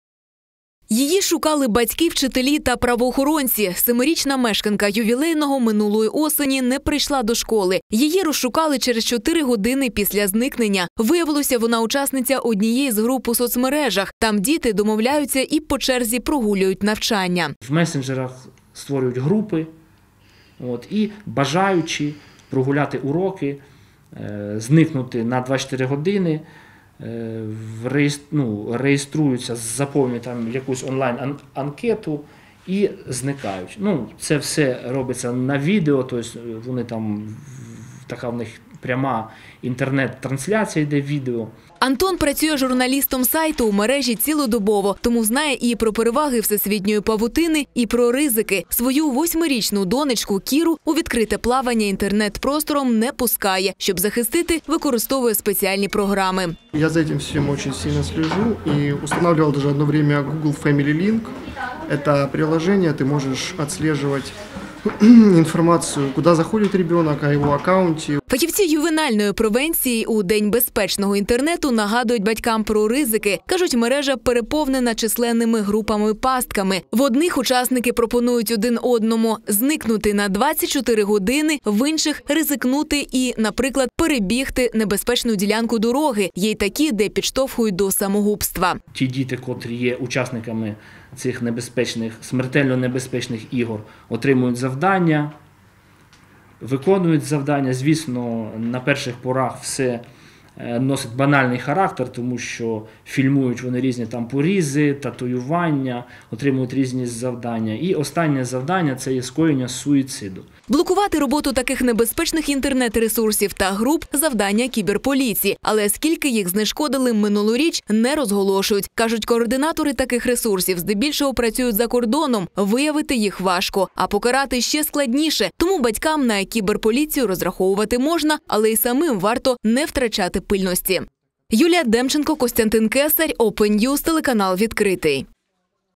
У Дніпрі відзначили День безпечного інтернету
Інформацію на будь який смак, різноманітні курси та розваги.  Все це  можна знайти в інтернеті. В той же час в мережі підстерігають кібер злочинці, інтернет залежність та численні групи ризику, до яких потрапляють і діти, і дорослі. Сьогодні у світі  -  День безпечного інтернету. Експерти  вкотре нагадали про онлайн- небезпеку та закликали батьків суворіше контролювати маленьких користувачів.